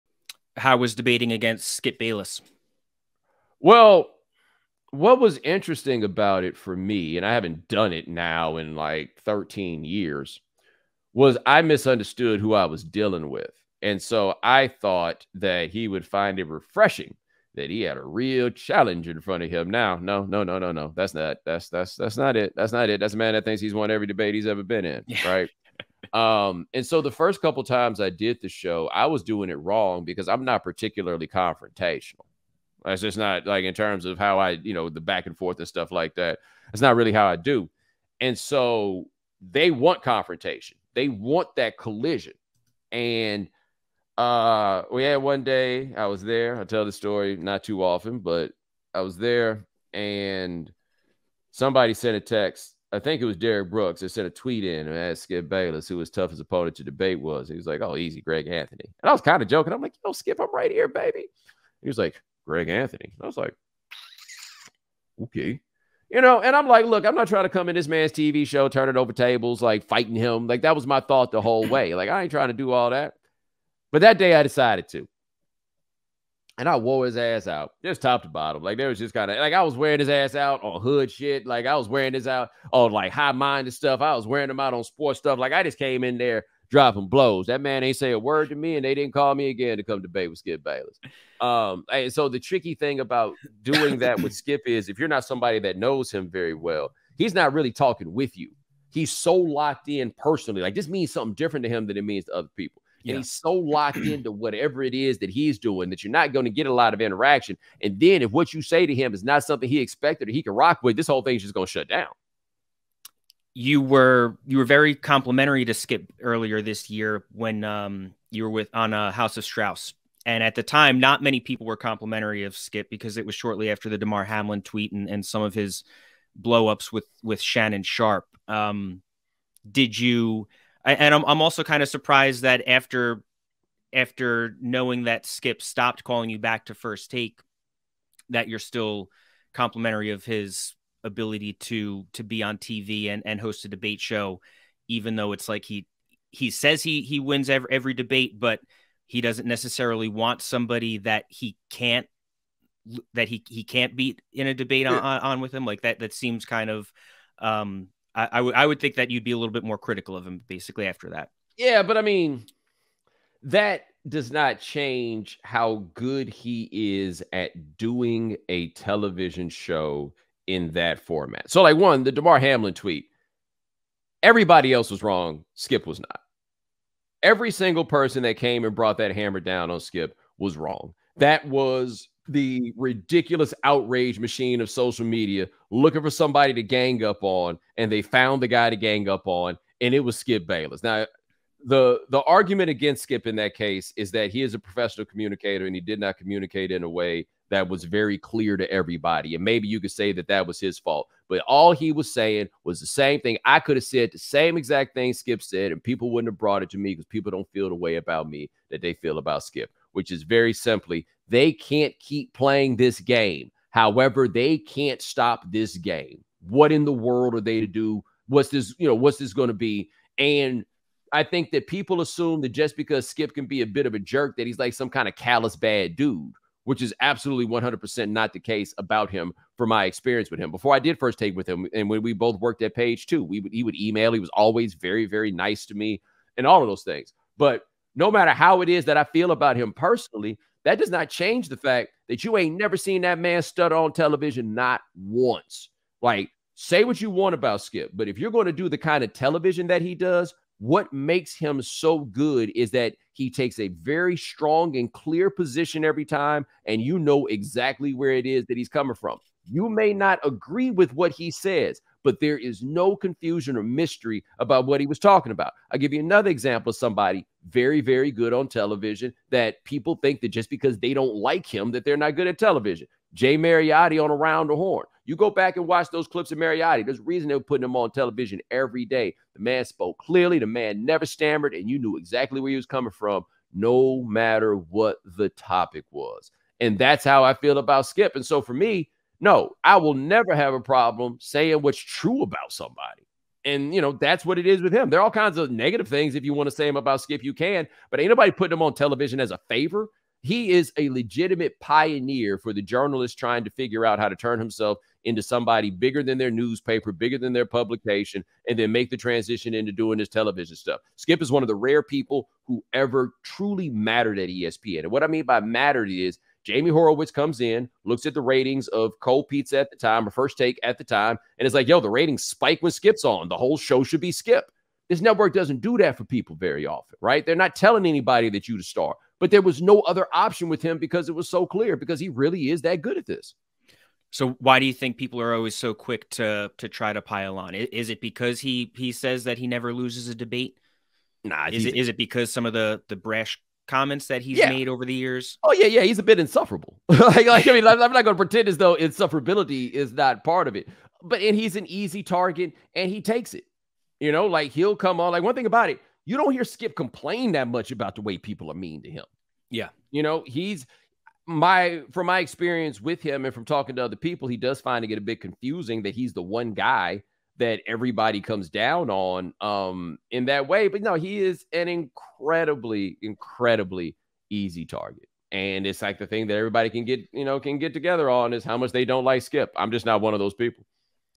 Bomani Jones talks debating against Skip Bayless
Bomani Jones pulls back the curtain about debating against Skip Bayless, defending Skip for his controversial Damar Hamlin tweet, and explains why Skip is so great on television.